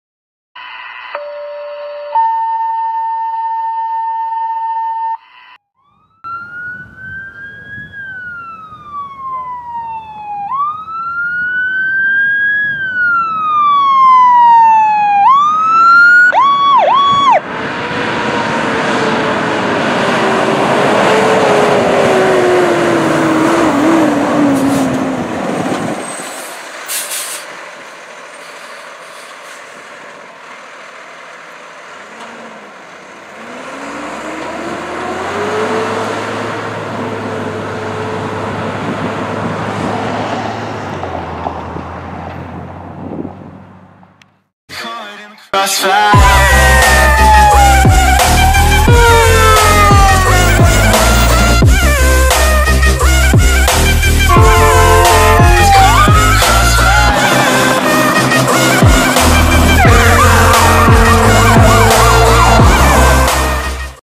Deer Creek Tanker 1 responding (Powercall) 12/16/18
I know, I know, it's not a lot of P-Call, but it is there 😂

Deer Creek Tanker 1 responding as Mutual Aid to a Structure Fire in Piedmont, using a little bit of Powercall.

***EVERY VIDEO ON THIS CHANNEL IS MINE AND IS UPLOADED TO THIS CHANNEL AND THIS CHANNEL ONLY***